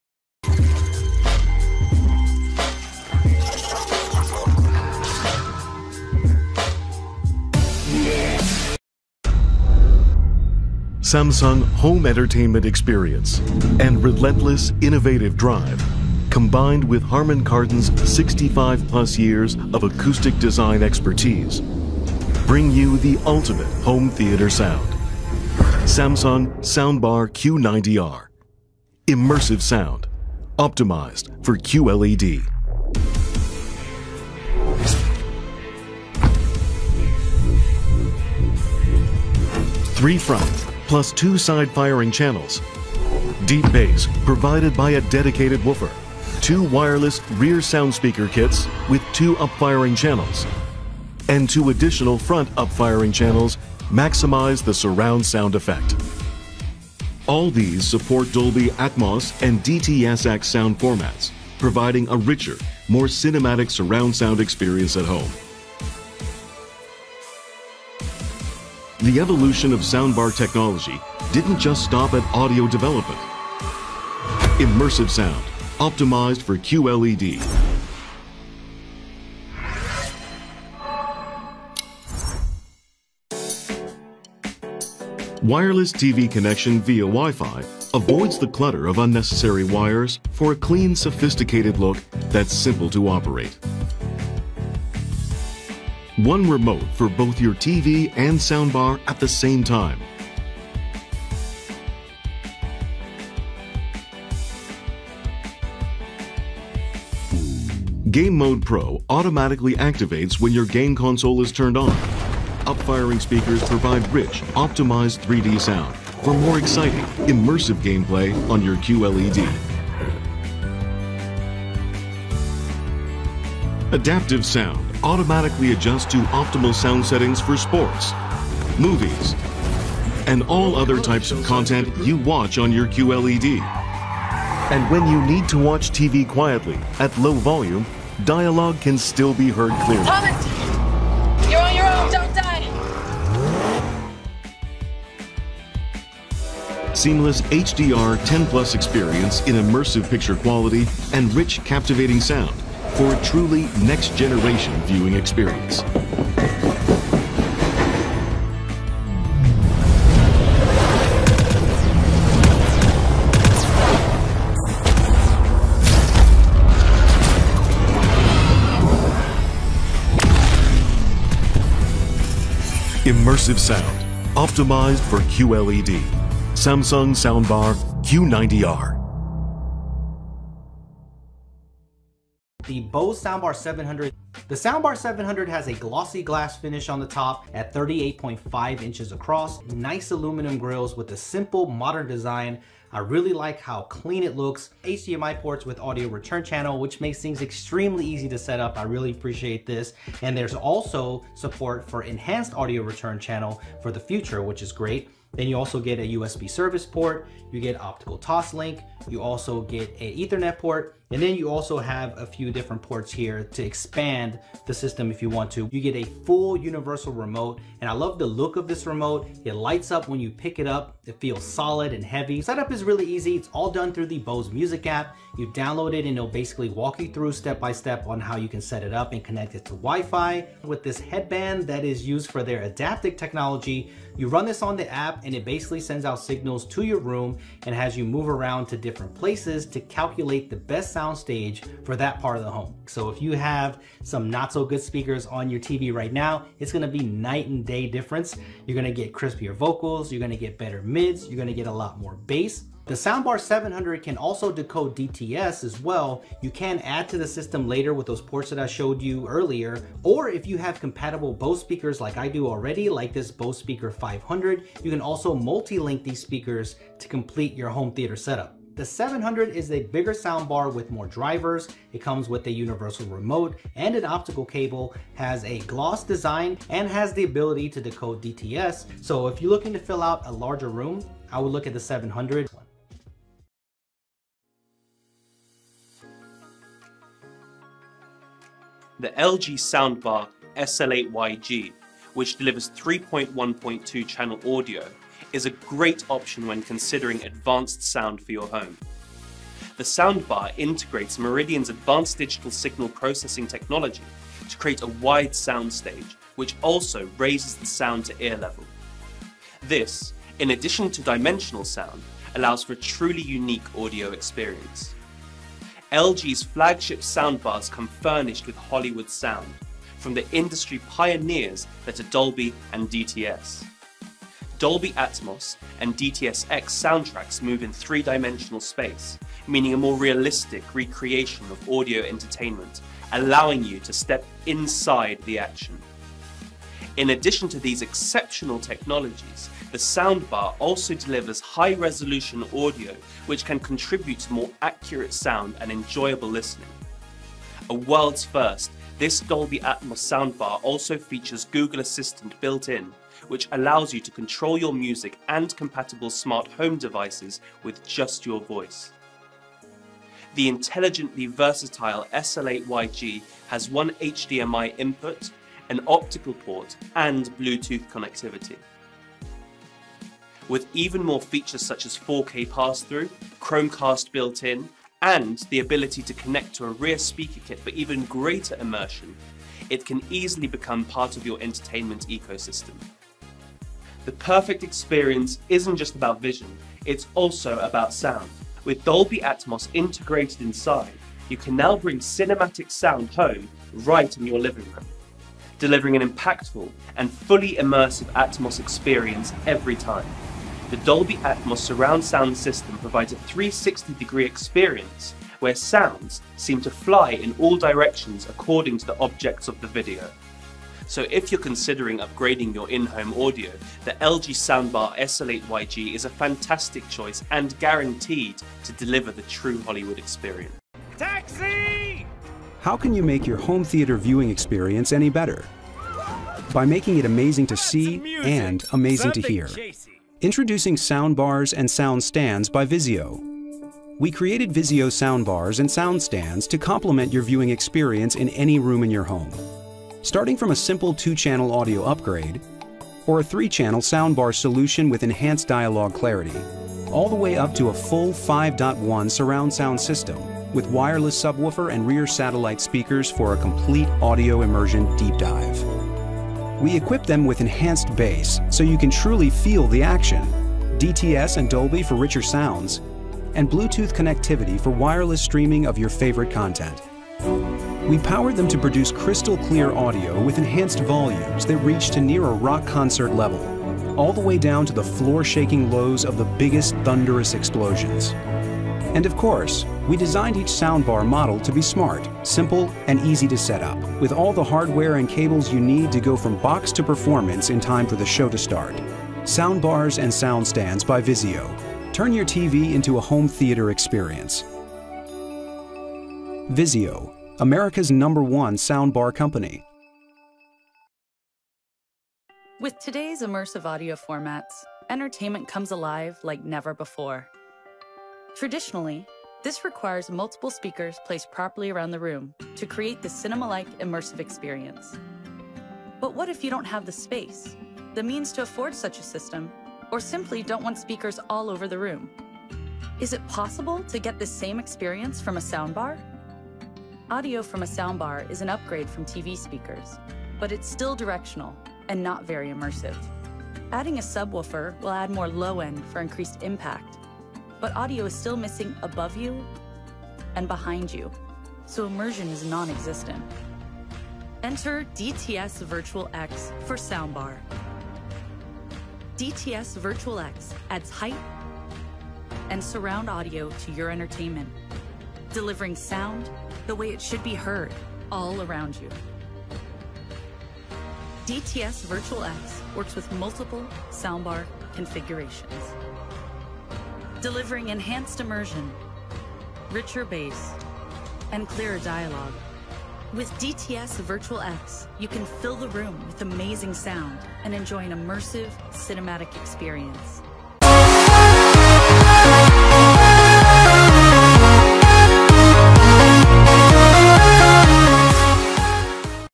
✅ 2020 Top 5 Best Soundbar | Samsung, Bose, LG, VIZIO
► Links to the Best Soundbar 2020 we listed in this video:

► 1 - Samsung Harman Kardon 7.1.4 Dolby Atmos Soundbar
► 2 - Bose Soundbar 700
► 3 - LG SoundBar SLY8YG
► 4 - VIZIO 2.1 Sound Bar SB3821
► 5 - LG SL5Y 2.1 Channel High Resolution Sound Bar